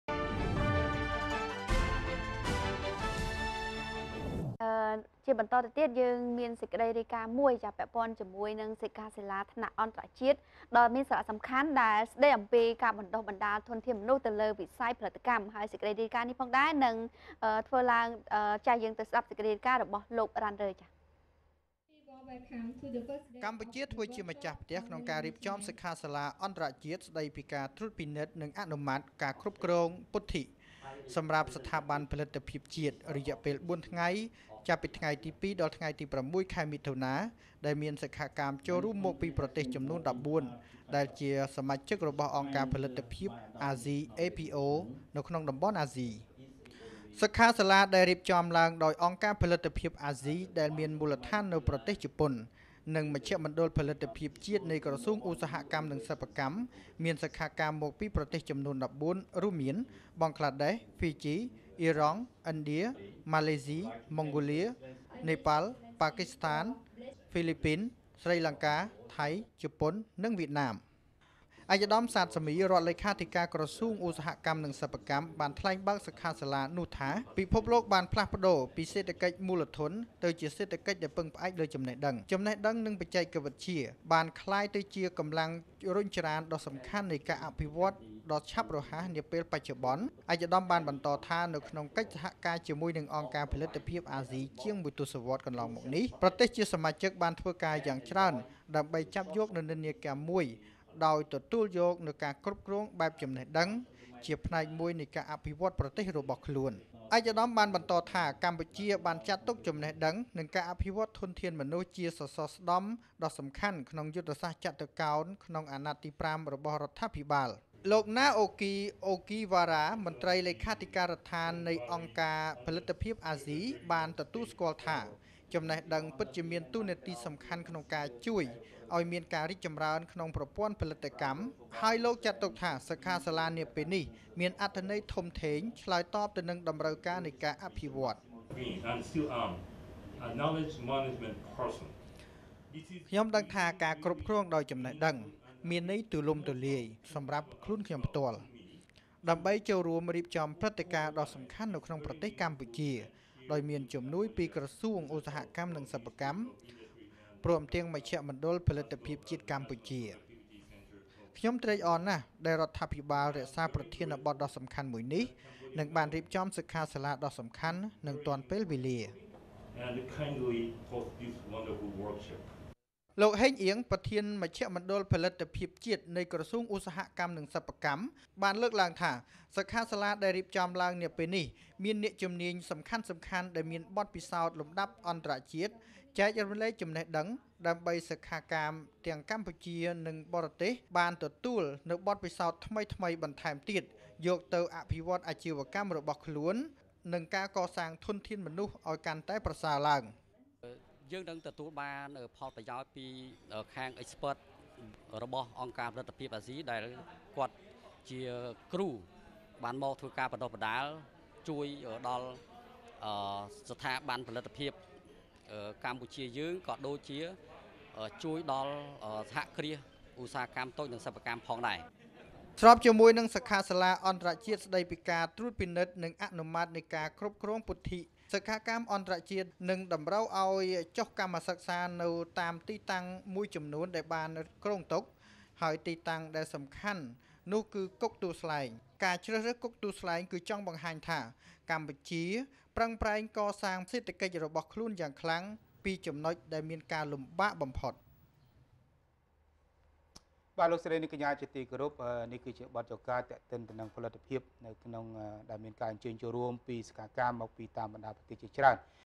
APO Workshop in Cambodia/CTN (3 & 6 June 2015) /Asian Productivity Organization
Cambodia Television Network (3 & 6 June 2015)/APO Workshop on Knowledge Management Adoption and Monitoring for NPOs/Asian Productivity Organization